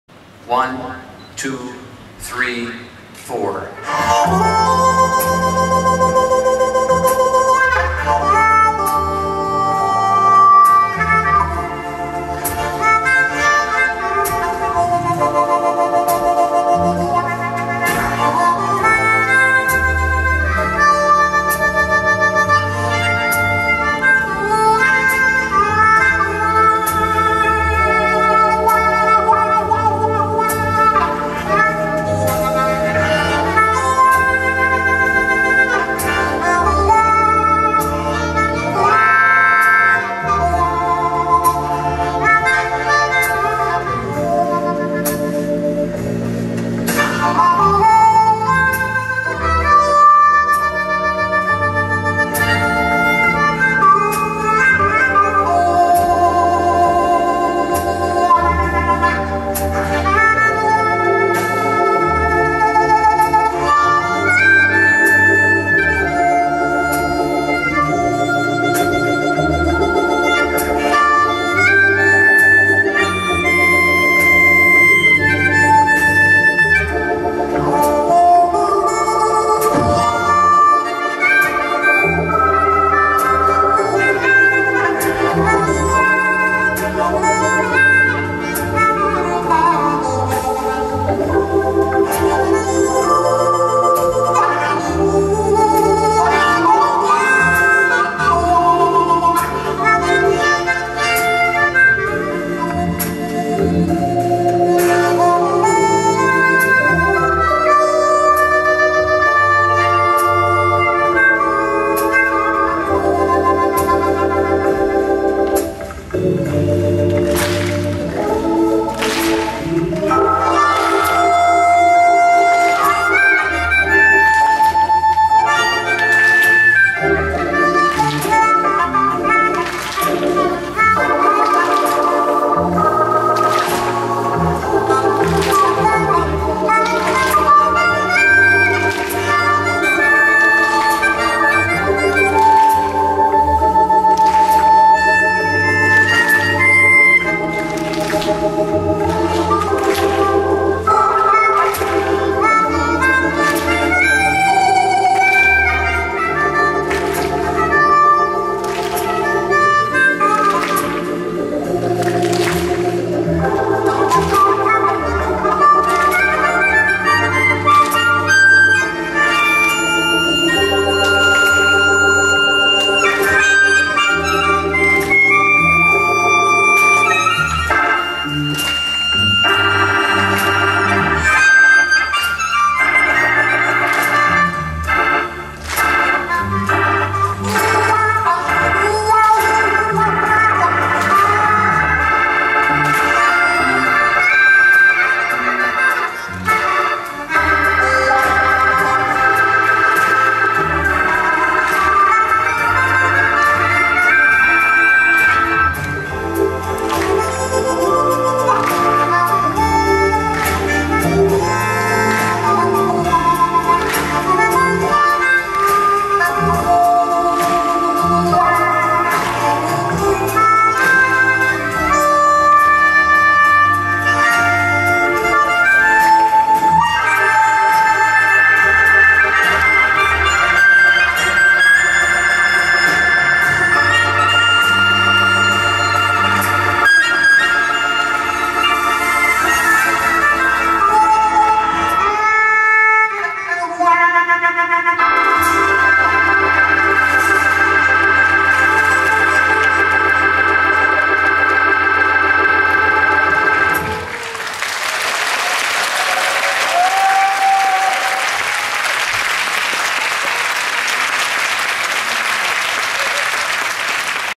The2ND SEOL International Harmonica Festival(JOE POWERS)2/4
2014/7/26The2ND SEOL International Harmonica Festival
Gala concert　Second day JOE POWERS
제 2 회 서울 국제 하모니카 페스티벌
갈라 콘서트